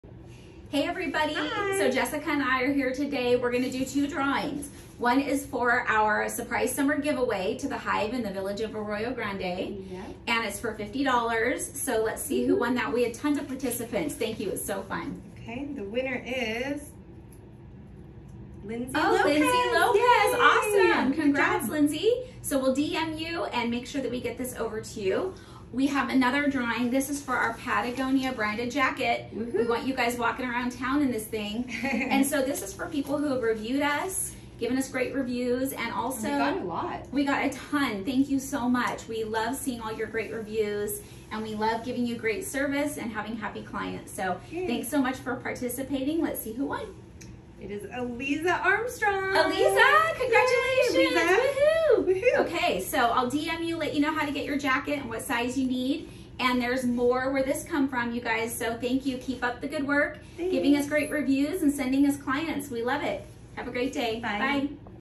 Giveaway Winners!
We had TWO GIVEAWAYS and it's time to announce the winners! 🎉⁣
Woo hoo! @lululopez you are the winner of the $50 giftcard to @thehive 💛🐝! Eleze Armstrong you are the winner of our CLRE Patagonia jacket! 👏🏻👏🏻 Congratulations to both of you!!! 🥳⁣
Thank you to everyone who entered and for all the amazing reviews you left for our team! 🙌🏻🙌🏻⁣ We're thankful for each and everyone of you 🤍
Christa Lowry⁣⁣⁣⁣⁣⁣⁣⁣⁣⁣⁣⁣⁣⁣
Coldwell Banker Premier Real Estate⁣⁣⁣⁣⁣⁣⁣⁣⁣⁣⁣⁣⁣⁣⁣⁣⁣⁣⁣⁣⁣⁣⁣⁣⁣⁣⁣⁣⁣⁣⁣⁣⁣⁣⁣⁣⁣⁣⁣⁣⁣⁣⁣⁣⁣⁣⁣⁣⁣⁣⁣⁣⁣⁣⁣⁣⁣⁣⁣⁣⁣
DRE01888396⁣⁣⁣⁣⁣⁣⁣⁣⁣⁣⁣⁣⁣⁣⁣⁣⁣⁣⁣⁣⁣⁣⁣⁣⁣⁣⁣⁣⁣⁣⁣⁣⁣⁣⁣⁣⁣⁣⁣⁣⁣⁣⁣⁣⁣⁣⁣⁣⁣⁣⁣⁣